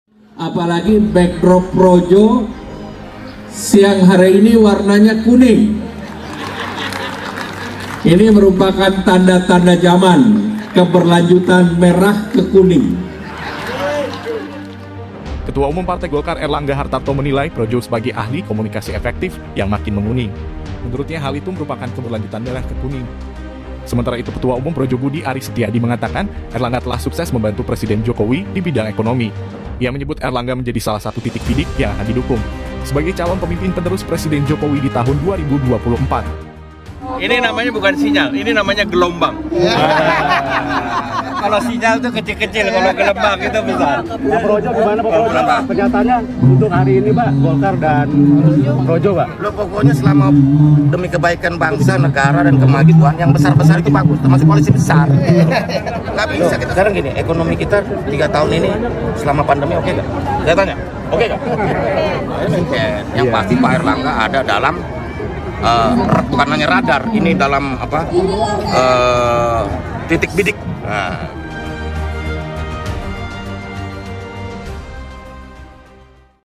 Projo Makin Menguning, Airlangga Hartarto: Keberlanjutan Merah ke Kuning
Ketua Umum Partai Golkar Airlangga Hartarto menilai kelompok sukarelawan Pro Jokowi (Projo) sebagai ahli komunikasi efektif yang makin menguning.

“Projo kita bilang sebagai ahli komunikasi efektif. Apalagi backdrop Projo siang hari ini warna kuning. Ini merupakan tanda-tanda jaman, keberlanjutan merah ke kuning,” kata Airlangga saat menghadiri acara Pasar Murah Projo di Stadion Mini Pagedangan, Kabupaten Tangerang, Sabtu (8/4).